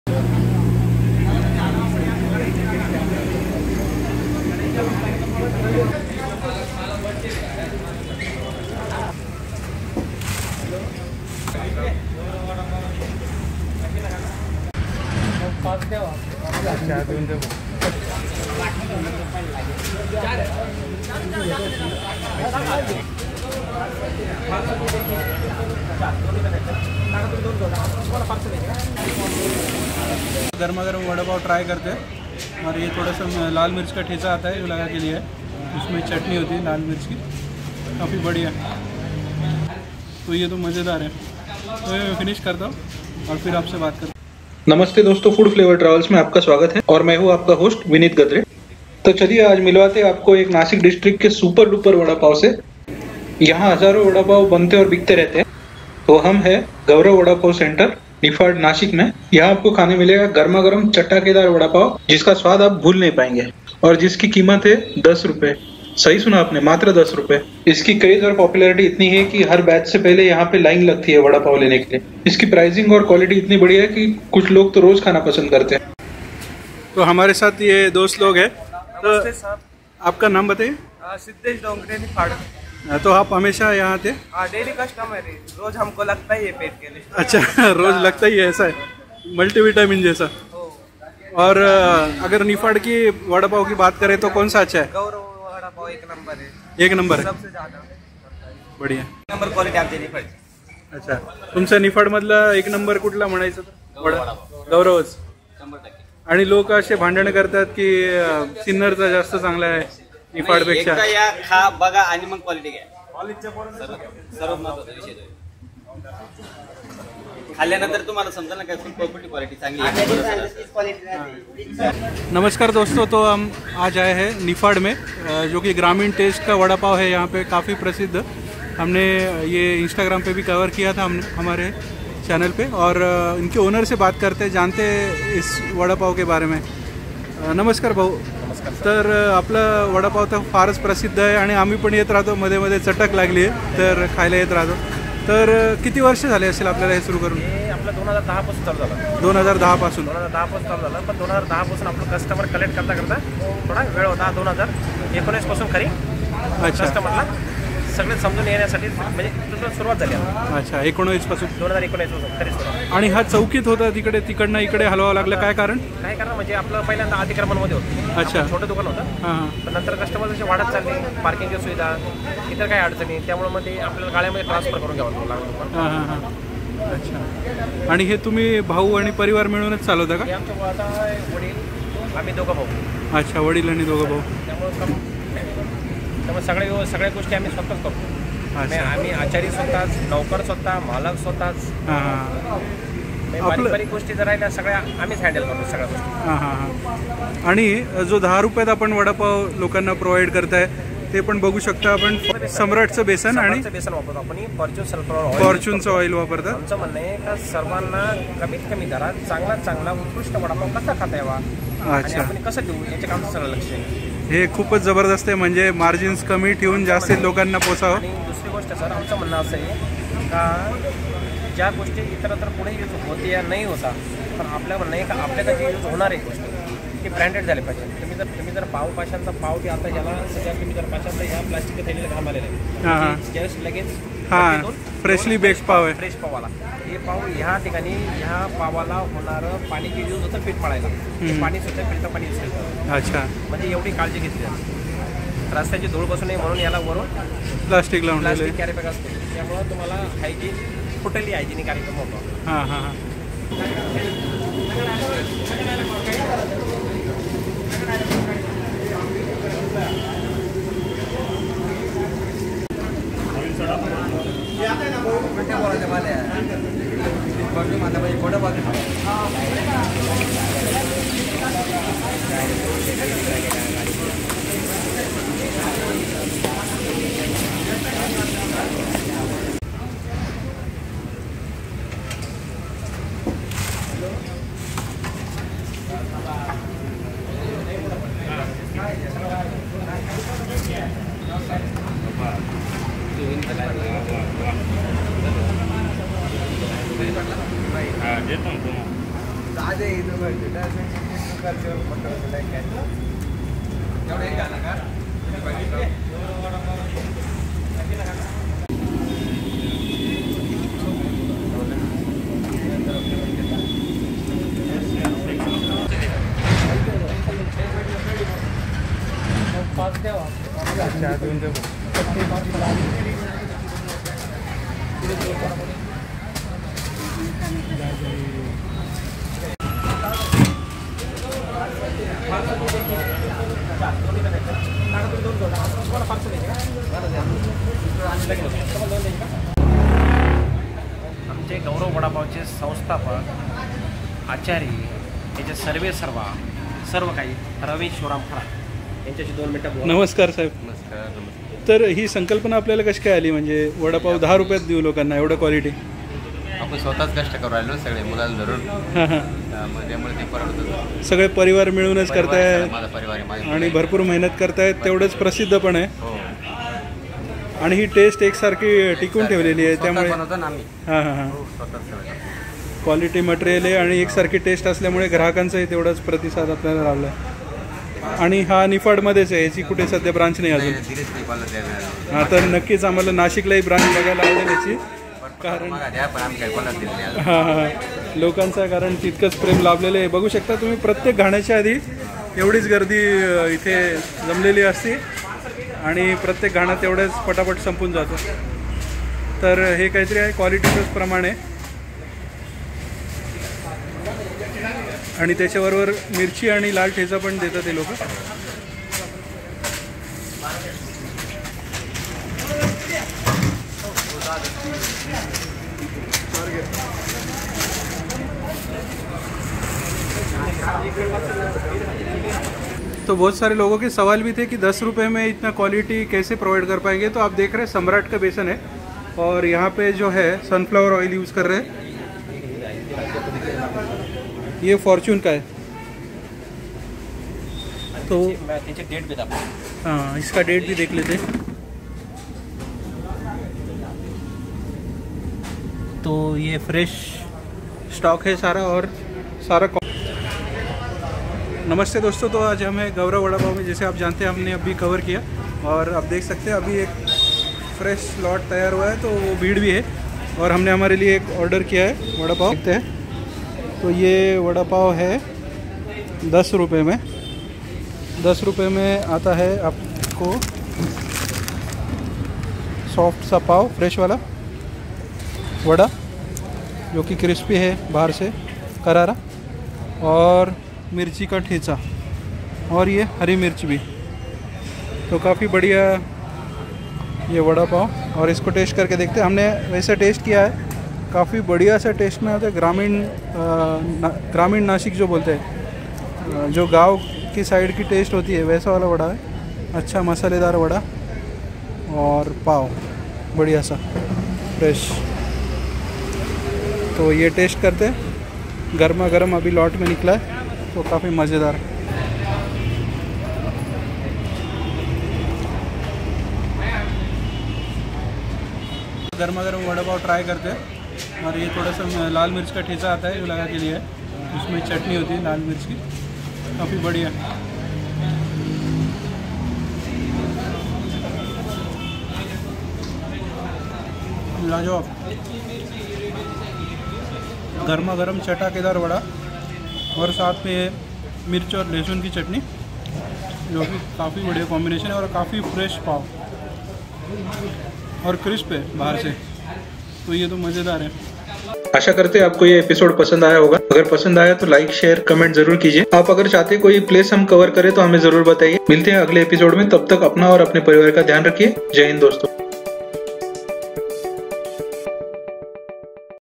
Super se bhi Uper wala Vadapaav @10 Rs only | Gaurav Vadapav
Gaurav Vadapav Niphad is one of the most famous Vadapaav of Nashik District
They sell very tasty and quality vadapaav at Rs 10/- only.There is a long queue of people before every batch of freshly fired vadapaav is out.
No doubt one of the best vadapav in Nashik

It is located on Nashik- Chatrapati Sambhaji Nagar Road near Niphad chowki.
People are crazy for this  vadaaav and some like to have daily one.
It is popular street food amongst College students,businessmen and locals in here.